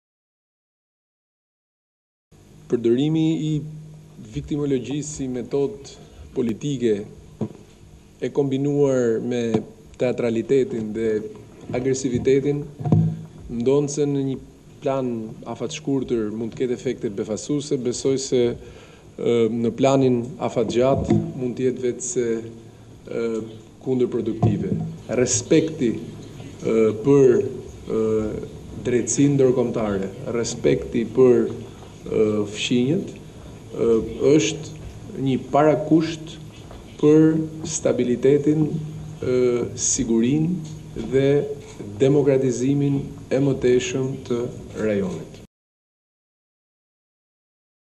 Konferenca - Bushati: Politika e Daçiç, viktimizim, kombinuar me teatralitetin dhe agresivitetin
Ministri i Punëve të Jashtme Ditmir Bushati priti sot në takim Ministrin e Punëve të Jashtme të Malit të Zi Srđan Darmanović, i cili po zhvillon një vizitë në Tiranë.
Bushati u pyet mbi deklaratat e homologut serb Daçiç  për të cilin tha se po
përdor viktimologjinë, por kjo metodë në një plan afatgjate do të jetë kunderproduktive.